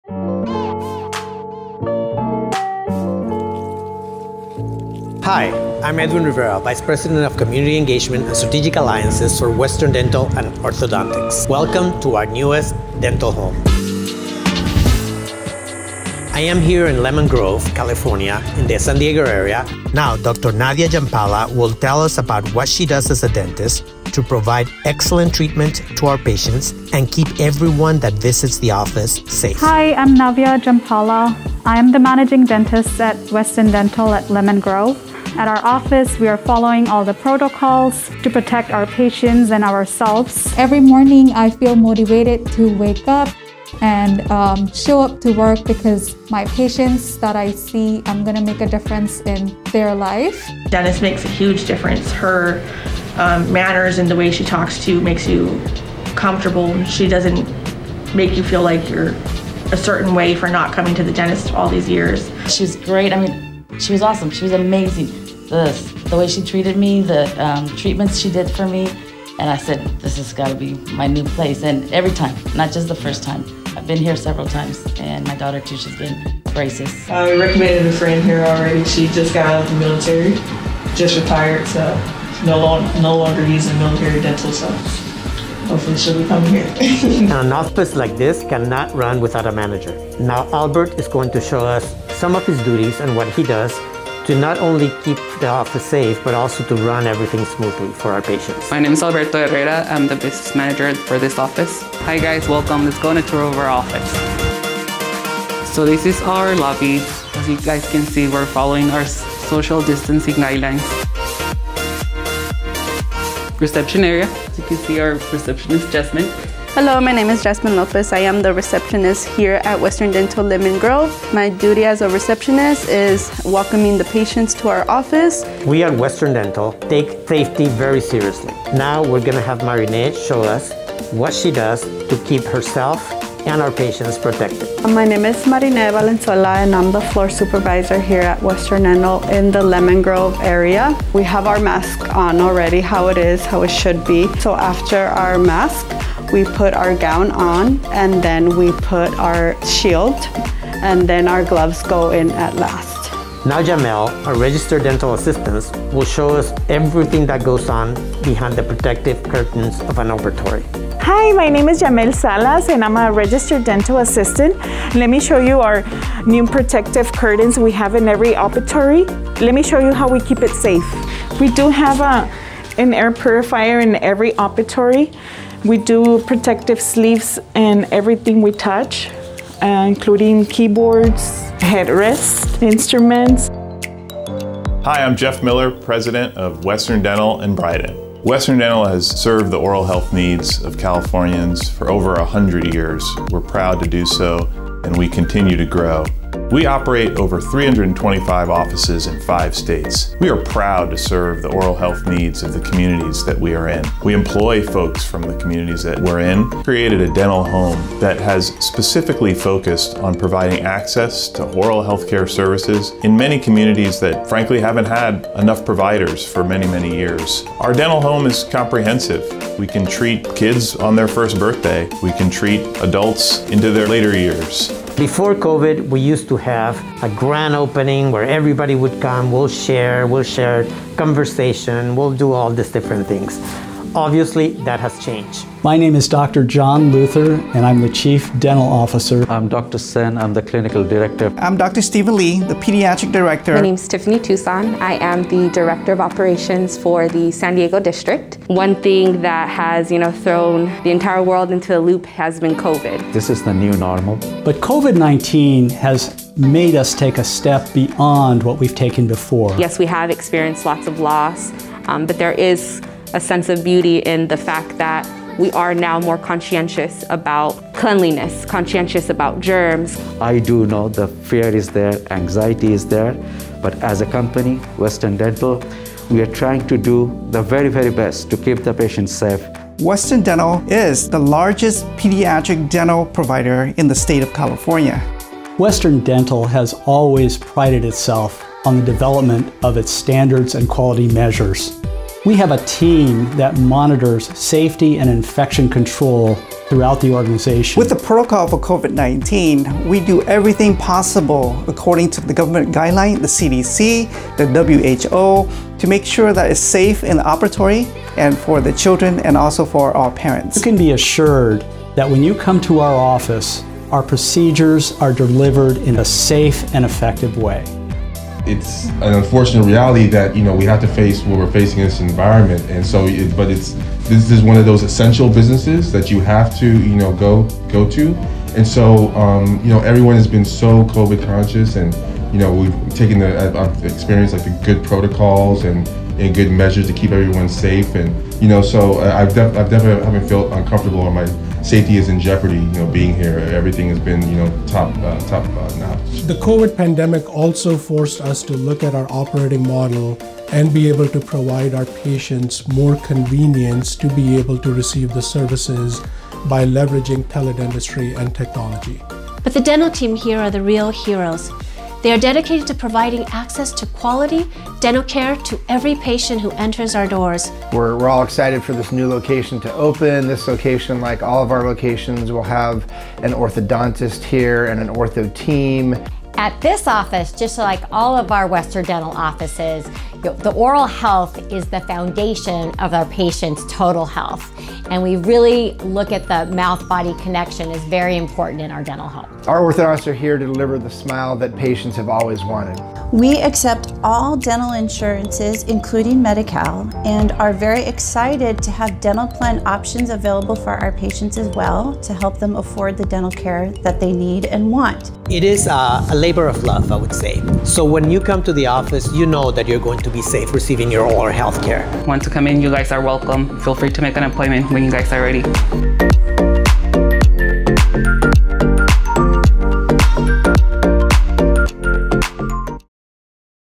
Your Newest Western Dental Home: Lemon Grove, CA (San Diego)
Edwin Rivera takes us inside the newest Western Dental office in Lemon Grove, Ca (San Diego Area). Here we meet some of the staff as well as some of our company's executives speak a bit about what we have done, and continue doing, in order to protect patients and staff during these times of health uncertainty.